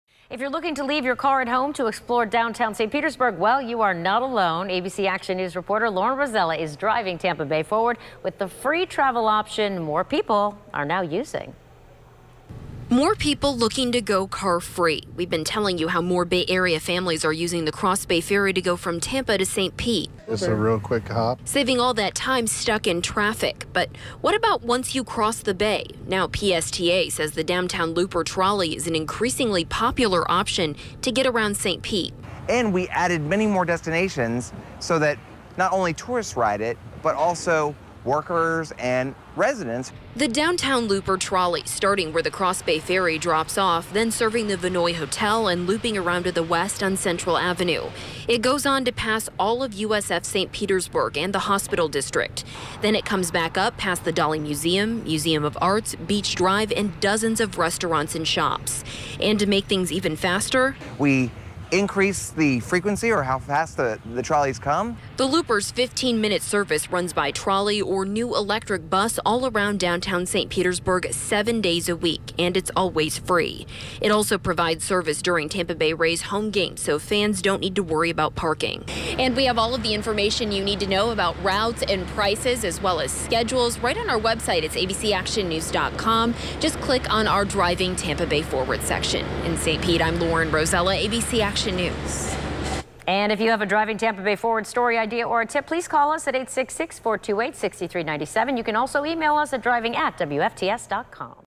PSTA: Downtown Looper ridership has doubled in one year
Ridership for St. Petersburg's Downtown Looper Trolley has doubled in ridership, from last year to this year, according to the Pinellas Suncoast Transit Authority.